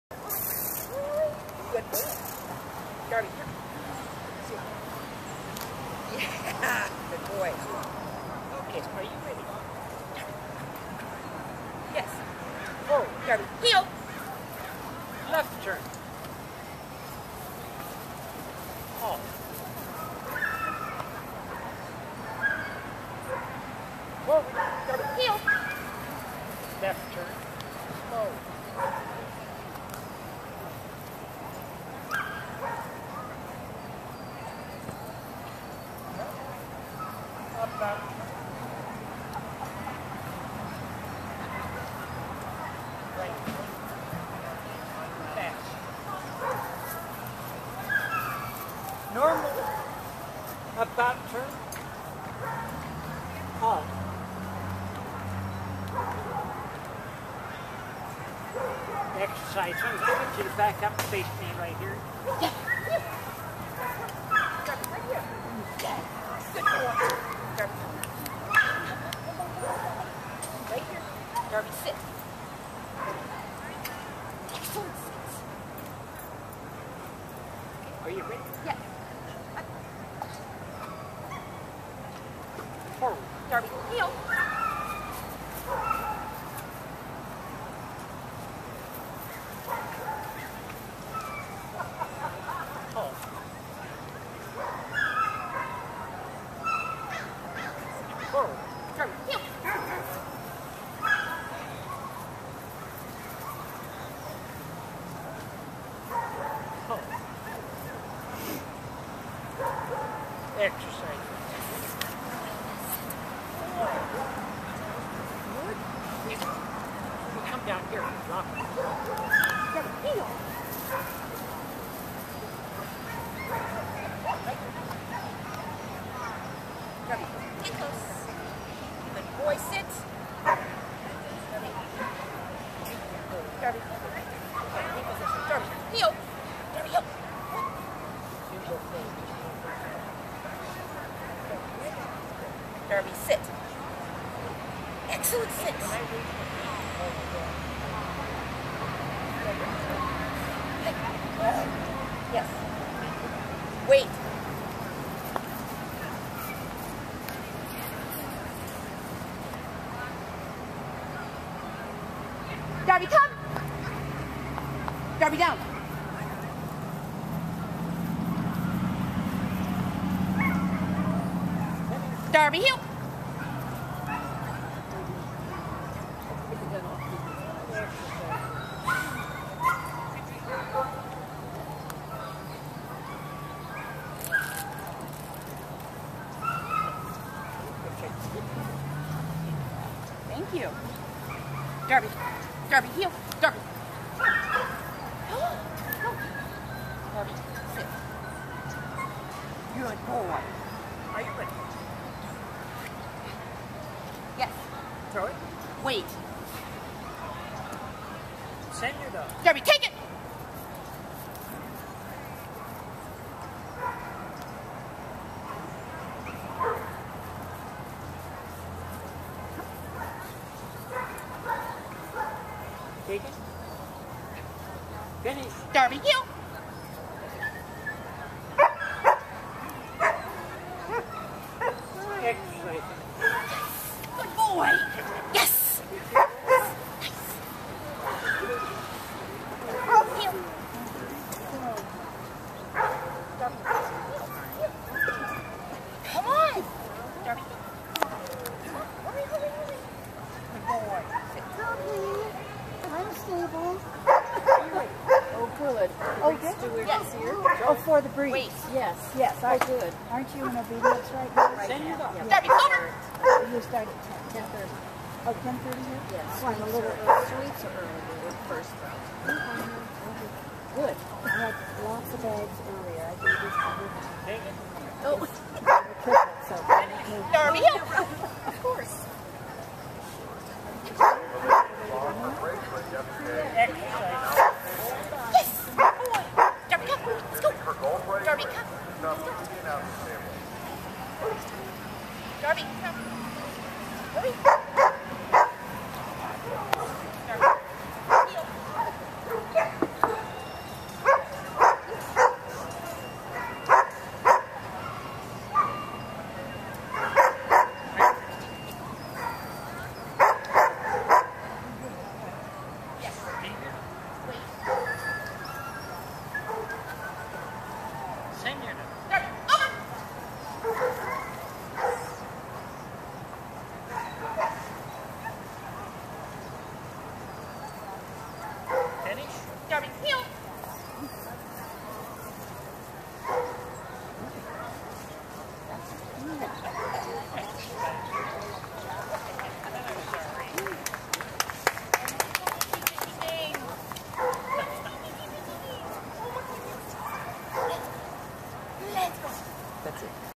Lisa and Darby Open A fall GCNC specialty
Lisa and Darby attempt Open A at the fall GCNC specialty in Pleasanton Ca. The cold wet grass  caused him to not drop on recall. Otherwise nice attempt.